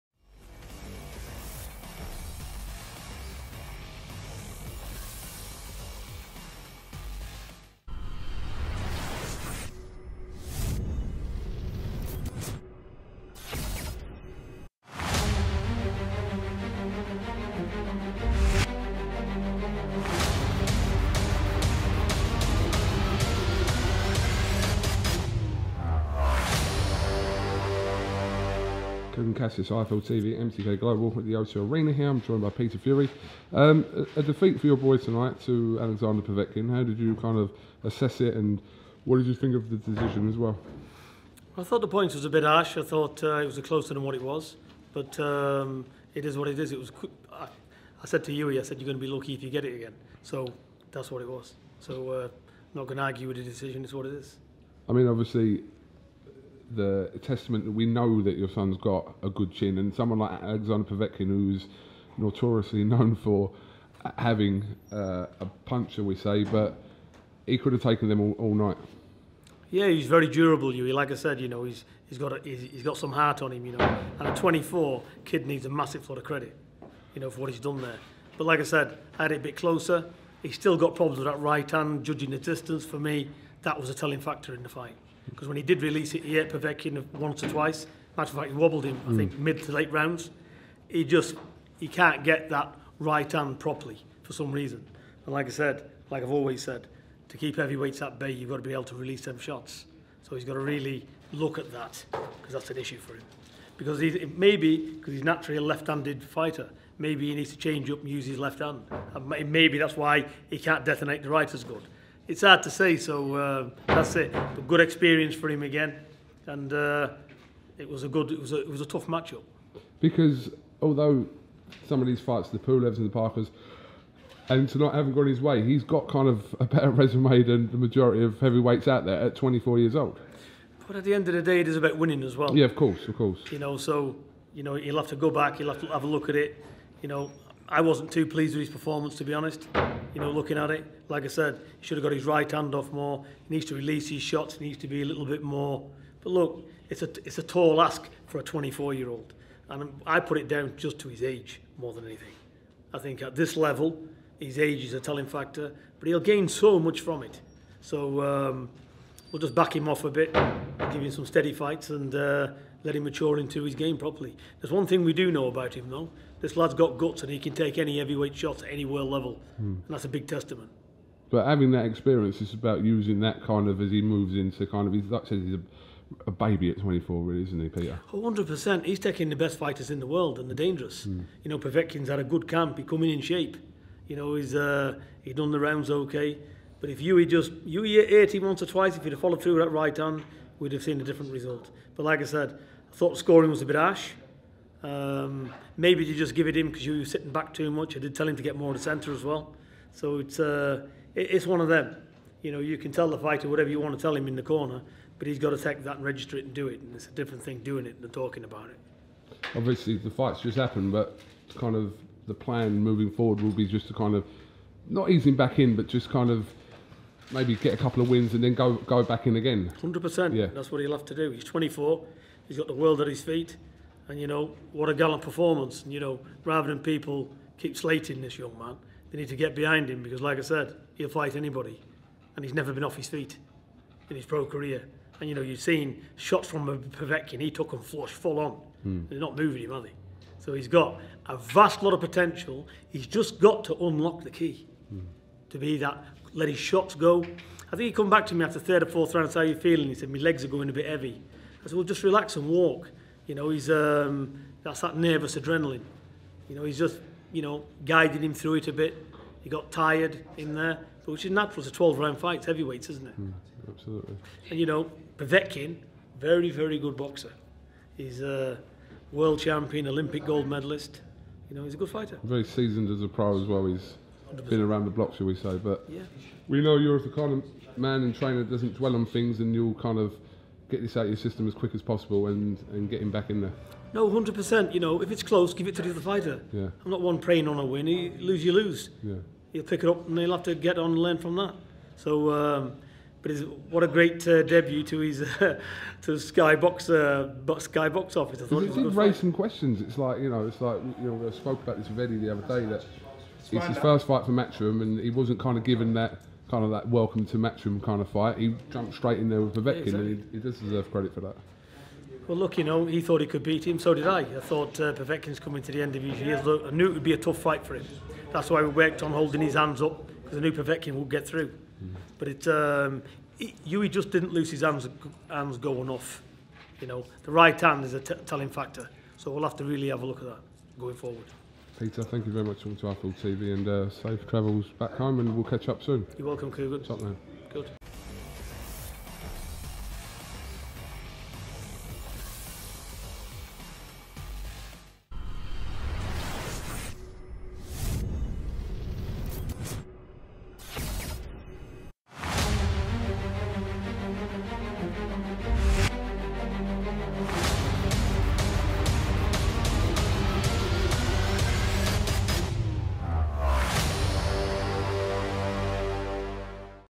PETER FURY IMMEDIATE REACTION TO HIS SON HUGHIE'S DEFEAT TO ALEXANDER POVETKIN / FURY-POVETKIN
PETER FURY IMMEDIATELY  REACTS TO HIS SON HUGHIE'S DEFEAT TO ALEXANDER POVETKIN / FURY-POVETKIN